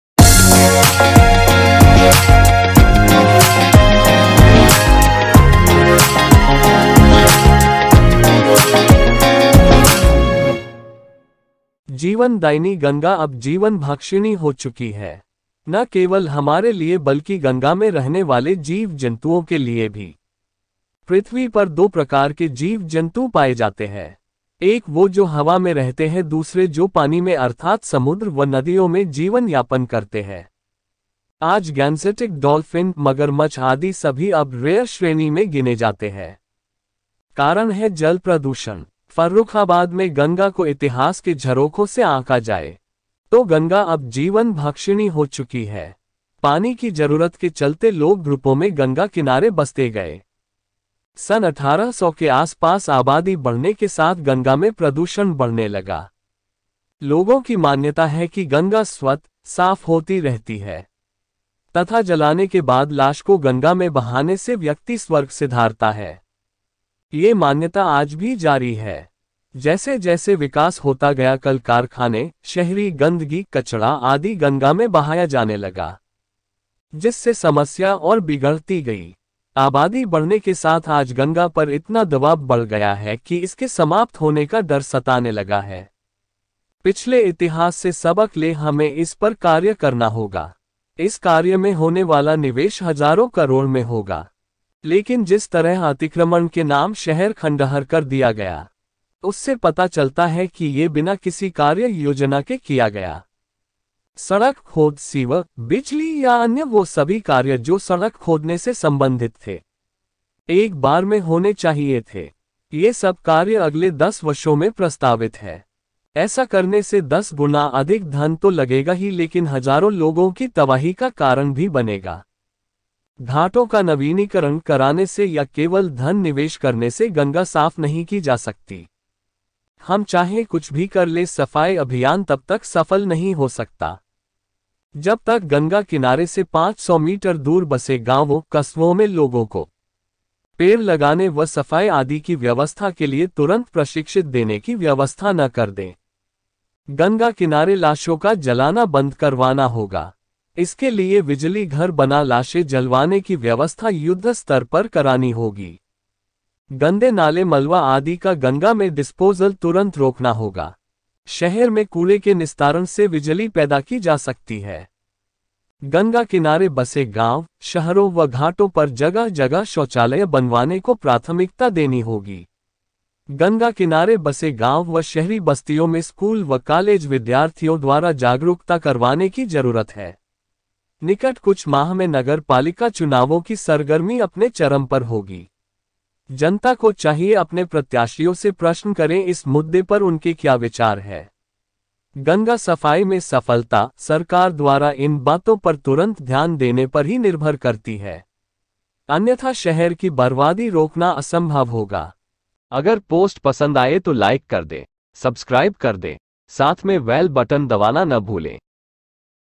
योजनाबद्ध कार्यन्वयन/प्रदूषण मुक्त गंगा ही विकास अन्यथा विनाष/history of farrukhabad
योजनाबद्ध कार्यन्वयन/प्रदूषण मुक्त गंगा ही विकास अन्यथा विनाष
जीवन दायिनी गंगा अब जीवन भक्षिणी हो चुकी है।न केवल हमारे लिये बल्कि गंगा में रहने वाले जीव जन्तुओं के लिये भी।पृथ्वी पर दो प्रकार के जीव जन्तु पाये जाते है।एक वो जो हवा में रहते है दूसरे जो पानी में अर्थात समुद्र व नदियों में जीवन यापन करते है।आज गैंजेटिक डाल्फिन,मगरमच्छ आदि सभी अब रेयर श्रेणी में गिने जाते है। कारण है जल प्रदूषण।
@glimpseonhistory @history channel